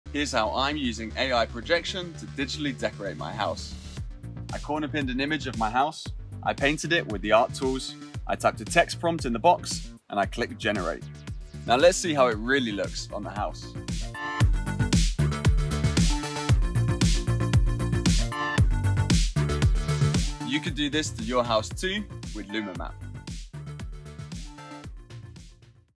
LumaMap | 🎃 Halloween decoration in a click
I digitally decorated my house for Halloween in a click with LumaMap

✨ LumaMap is an all-in-one projection mapping tool, powered by AI. Create stunning, bespoke visuals from just an image and a text prompt.